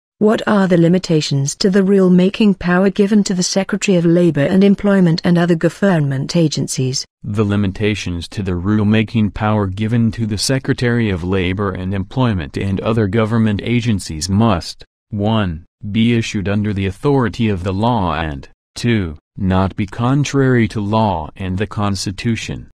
What are the limitations ..... given to the Secretary of Labor and Employment
The limitations to the “rule‐making power” given to the Secretary of Labor and Employment and other government agencies must:
1. Be issued under the authority of the law and;
2. Not be contrary to law and the Constitution